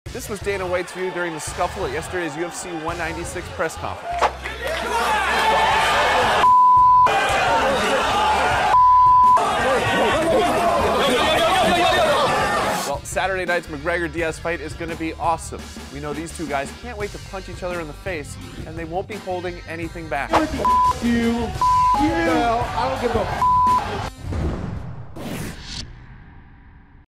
Dana White’s view during McGregor-Diaz scuffle was pretty intense
Things got heated at Thursday’s UFC 196 press conference and Dana White had the best view of it all.